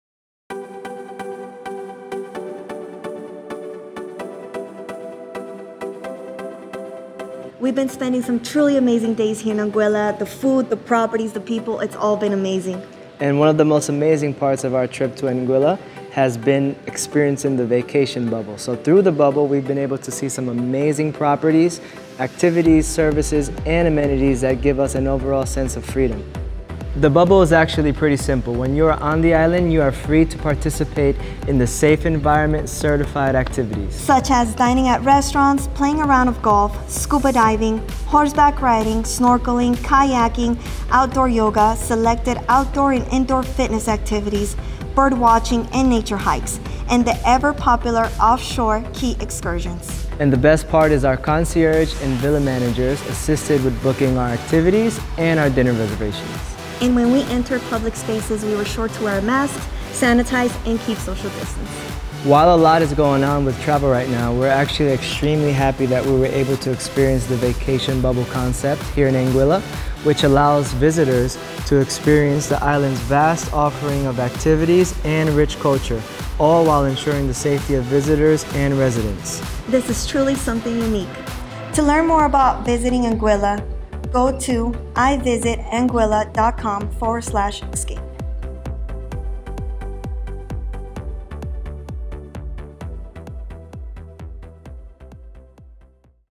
Anguilla's Bubble Experience. - Jeremy & Angie
Wondering what it's like to visit Anguilla and experience the bubble?  This video captures Anguilla's bubble experience.